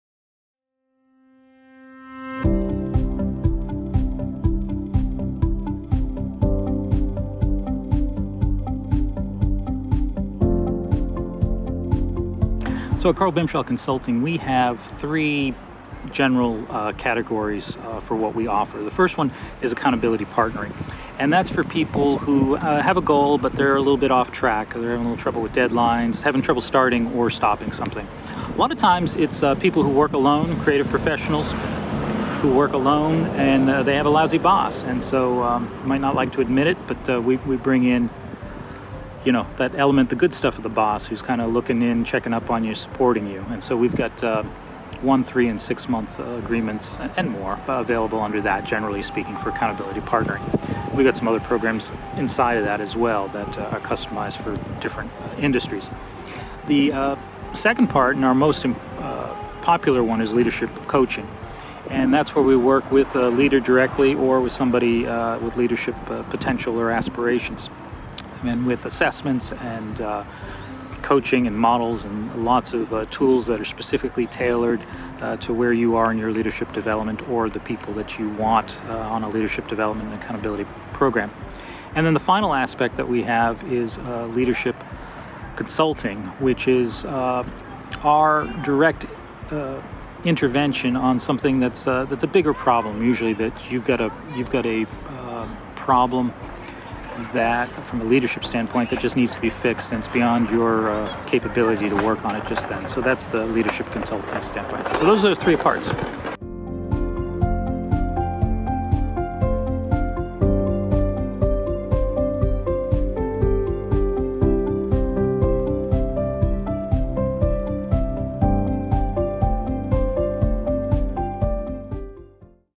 Our Big Three
Question: "Tell me about Karl Bimshas Consulting?"

At Karl Bimshas Consulting, we have three general categories for what we offer.

The first one is accountability partnering, and that's for people who have a goal, but they are a little bit off track. They're in a little trouble with deadlines, having trouble starting or stopping something. Many times, it's people who work alone, creative professionals who work alone and have a lousy boss. They might not like to admit it, but we bring in that element -- the good stuff -- of the boss who's looking in, checking up on you, supporting you. We've got one, three, and six-month agreements — and more, available Accountability Partnering. We've some other programs inside of that as well that are customized for different industries.

The second part, and our most popular one, is Leadership Coaching. That's where we work with the leader, directly or with somebody with leadership potential or aspirations, and we come in with assessments and coaching, and models, and lots of tools specifically tailored to where you are in your leadership development -- or the people that you want on a leadership development accountability program.

The final aspect that we have is, Leadership Consulting, which is our direct intervention on something that's a bigger problem, usually, that you've got. You've got a problem that, from a leadership standpoint, needs to be fixed, and it's beyond your capability to work on it. So that's the leadership consulting standpoint.

Those are the three parts. Learn more by going to LearnfromKarl.com